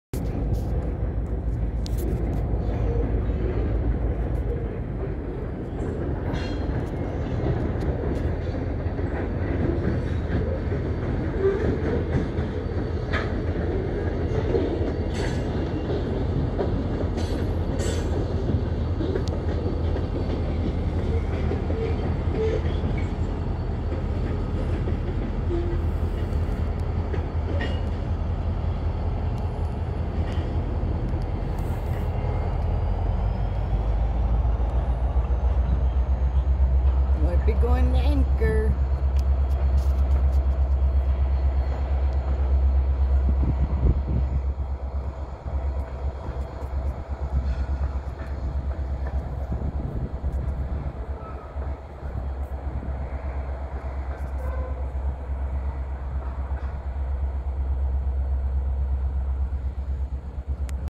Norfolk Southern, Headed to Anchor Glass Cnt. for pick up.
Grandma at Eldridge Park
Train passing through headed to pick up Anchor Glass.